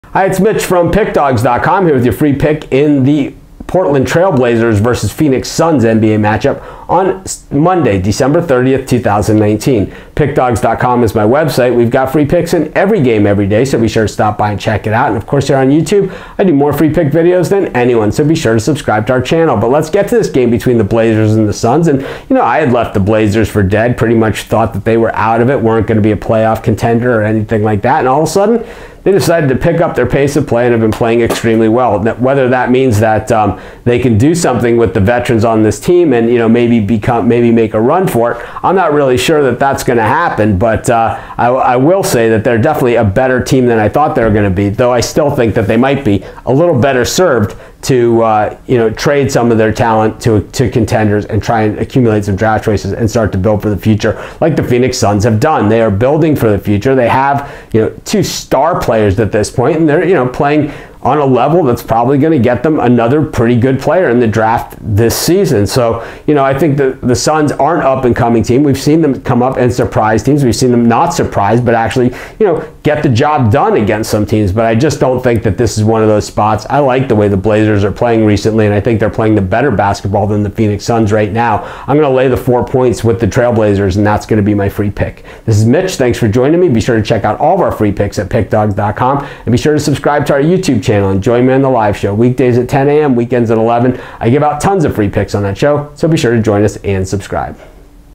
Phoenix Suns vs Portland Trail Blazers 12/30/19 Free NBA Pick and Prediction NBA Betting Tips
The Portland Trail Blazers host the Phoenix Suns in Monday NBA basketball action. Get Mitch's NBA Pick and Prediction for this game.